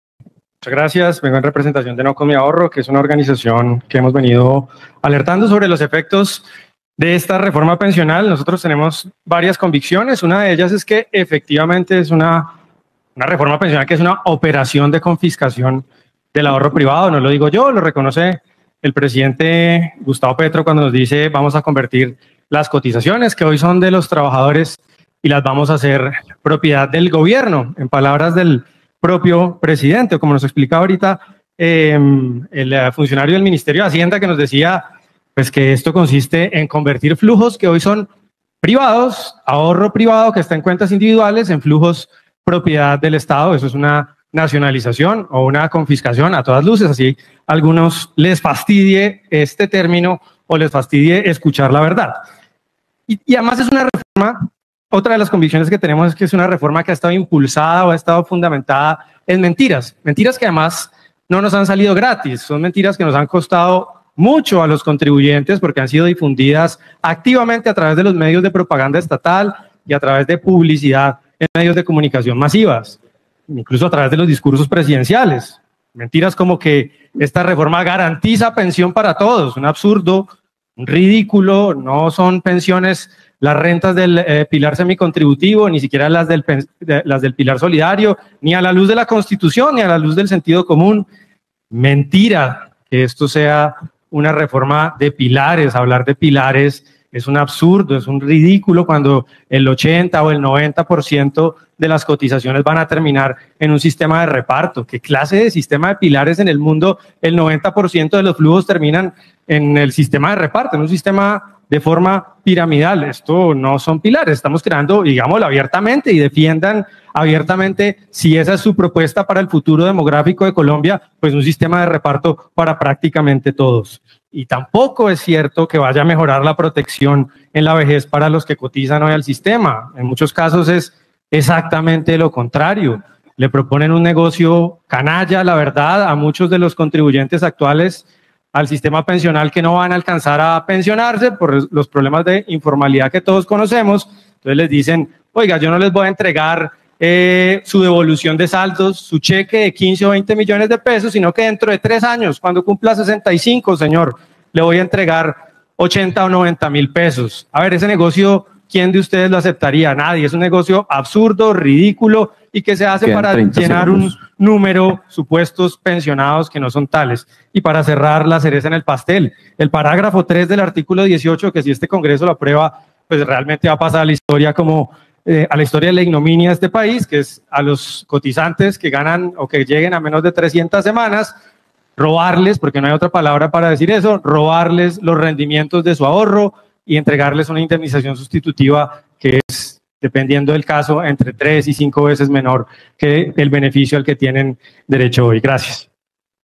La reforma pensional de Petro es una confiscación al ahorro de los colombianos: Julio César Iglesias
Escuche el programa en directo de Lunes a Viernes. Escuchanos en Tunein, Facebook, Youtube,